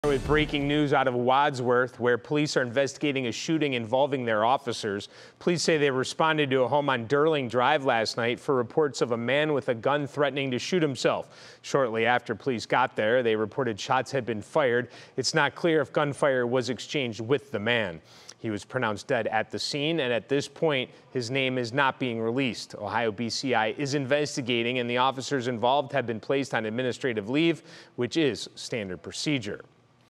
Man dies in Wadsworth shooting: Involved officers placed on leave as Ohio BCI investigates
We’re following breaking news out of Wadsworth where the Ohio Bureau of Investigation responded to a deadly shooting in the 200 block of Durling Drive.

Wadsworth police say it was around 9:02 p.m. Tuesday when officers responded to the scene “in response to a report of a male subject armed with a handgun and threatening suicide in his back yard.”

Shortly after police arrived at the residence, officers reported that gunshots had been fired. The press release from Wadsworth police, however, does not make it clear if gunfire was exchanged with the man.

Police say the man was pronounced dead at the scene. His name is being withheld at this time.

“It is the Wadsworth Police Department’s policy to have the Ohio Bureau of Criminal Investigation conduct the investigation of any officer-involved shooting incident,” according to a press release from Wadsworth police. “As such, BCI was requested and they responded to the scene to begin the investigative process.”